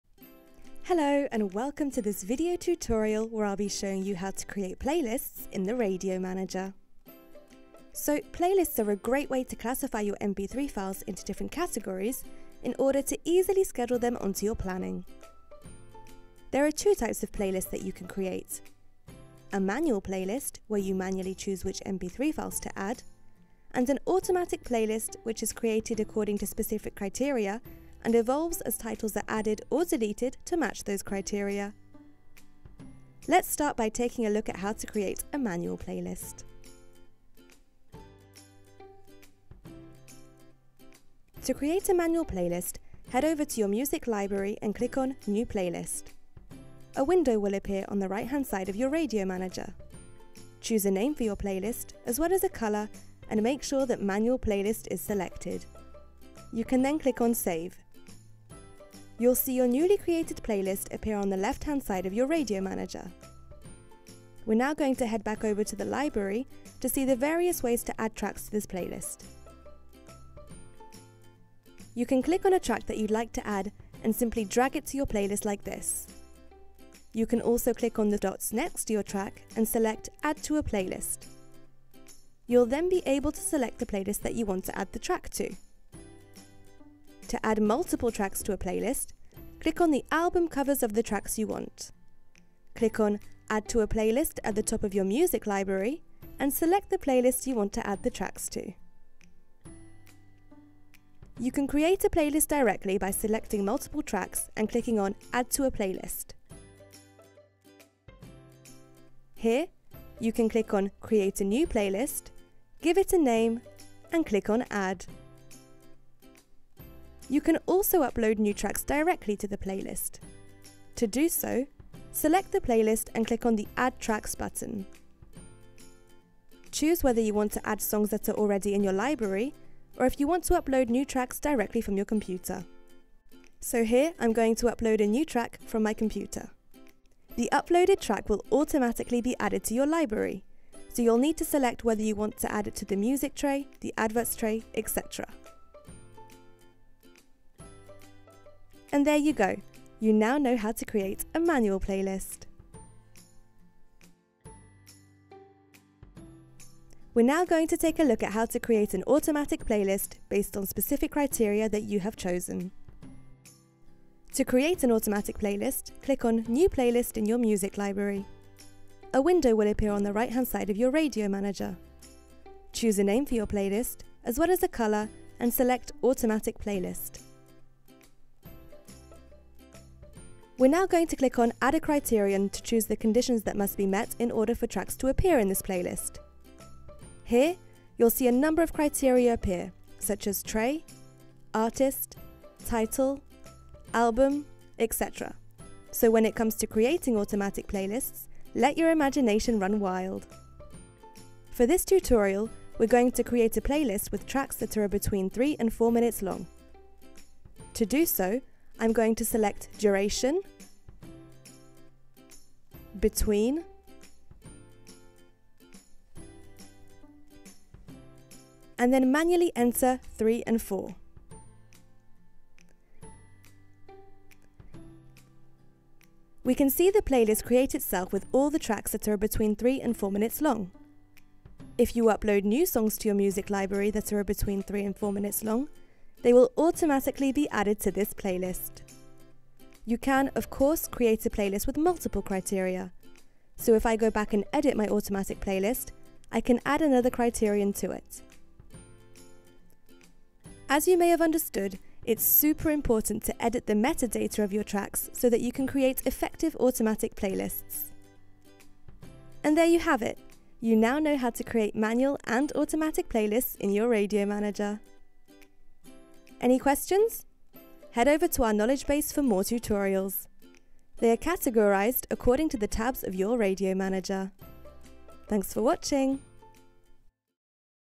Create Playlists in the Radio Manager
Discover how to easily create manual and automatic playlists in your Radio Manager!

📻Don't have a radio station yet? Try your free 14-day demo today